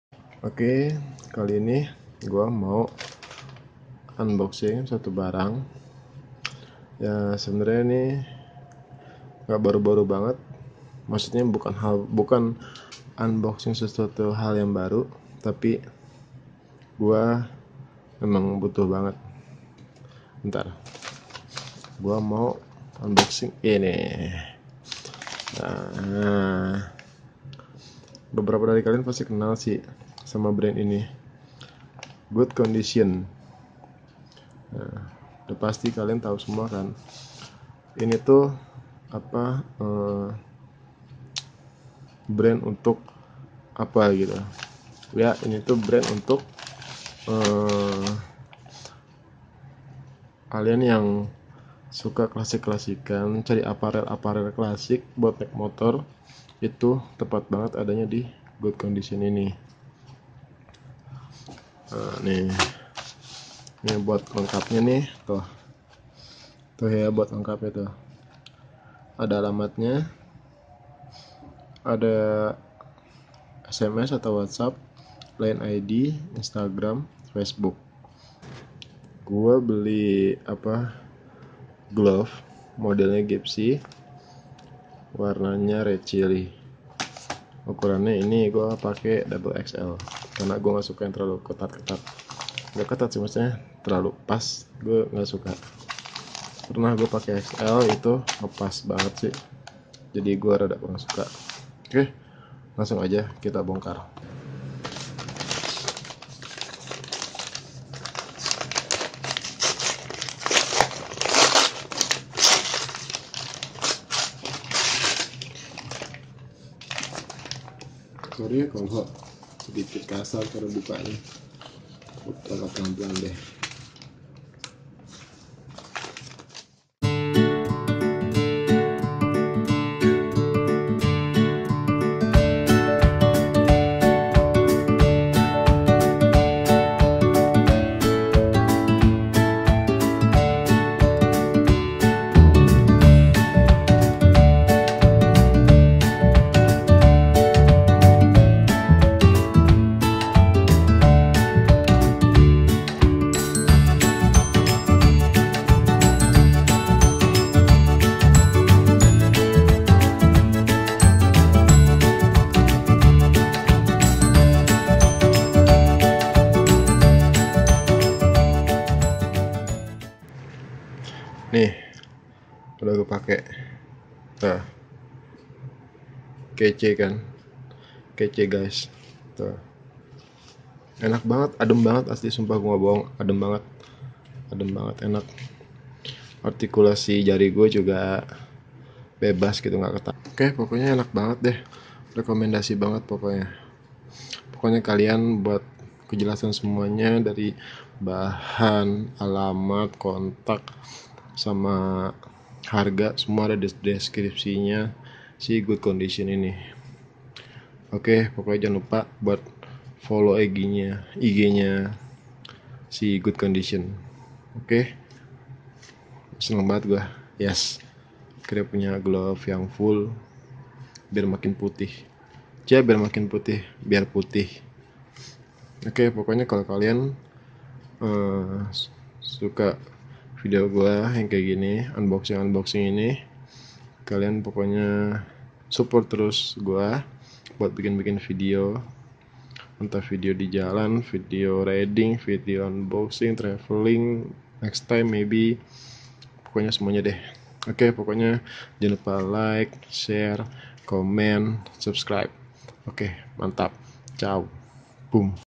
Unboxing Classic Gipsy Gloves
halo semua, kali ini gw mau unboxing sebuah glove yg bisa dibilang sekarangjadi glove yg kekinian di dunia permotoran

PS: bukan endorsan

follow juga IG gw : @drneoo

happy shoping guysss :)